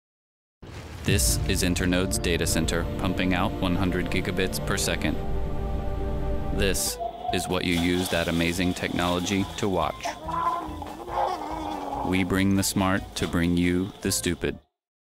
We bring the smart to bring you the stupid – 15sec TVC - Internode
Internode's data centre pumps out 100 gigabits per second so that you can watch videos like this.